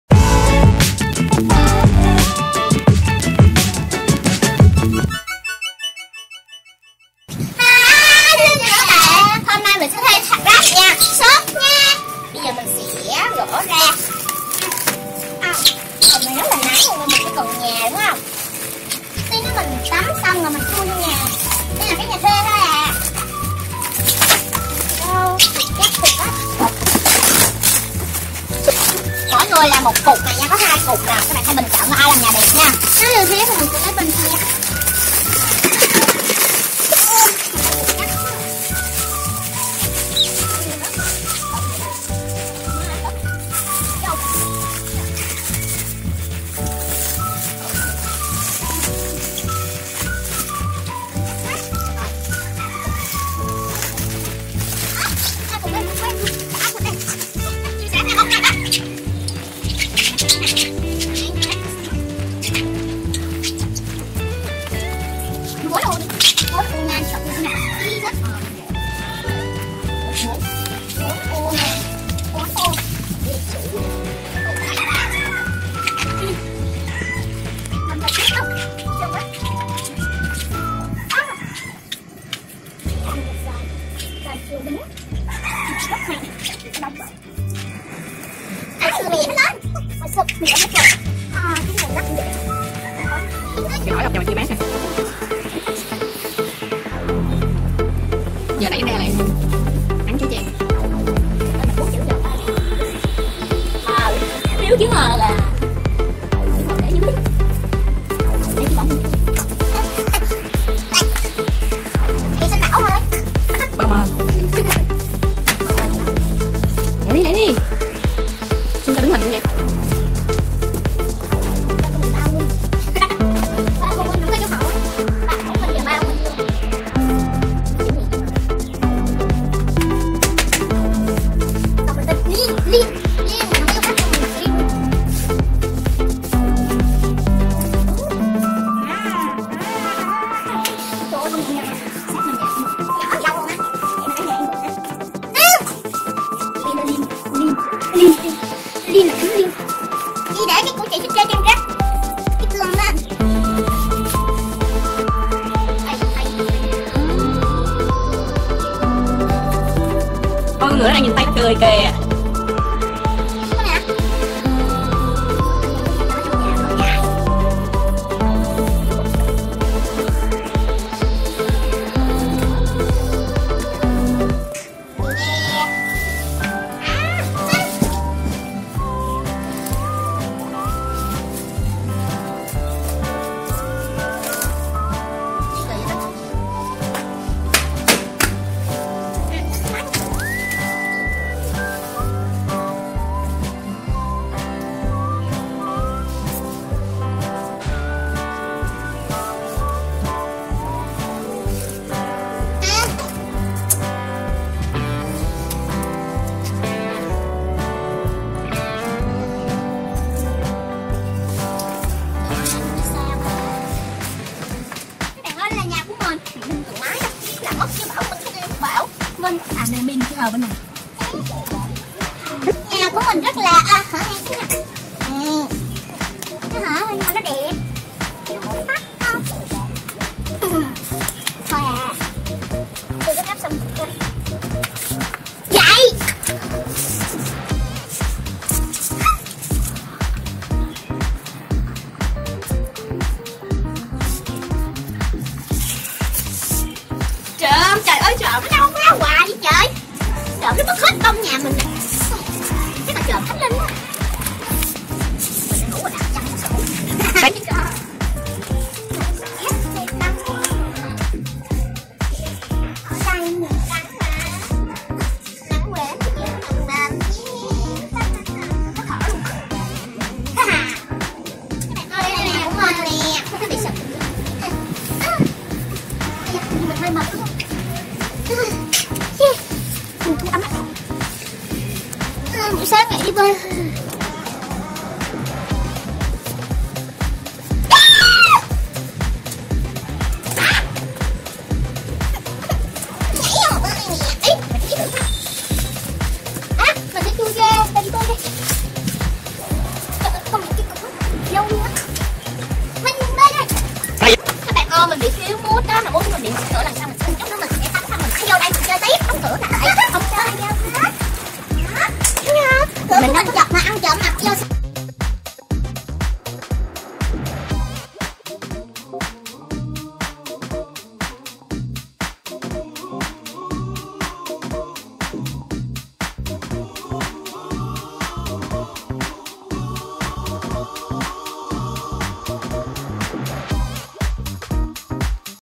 Trò chơi thi làm nhà bằng xốp chữ, ai làm đẹp và nhanh hơn? | Bao Minh Channel
Hi, chào các Anh chị & các bạn,
Các bạn có nhớ xe xốp mà mình dùng chơi cooking & cắm trại không ? Hôm nay, mình khui ra để thi làm nhà với Khánh Linh nhé, tưởng đơn giản mà không giản đơn chút nào nha các bạn.
Các bạn hãy tập làm thử cho vui nha, hy vọng mọi người thích video này nhé.
Cảm ơn các anh chị & các bạn rất nhiều.
LOVE YOU.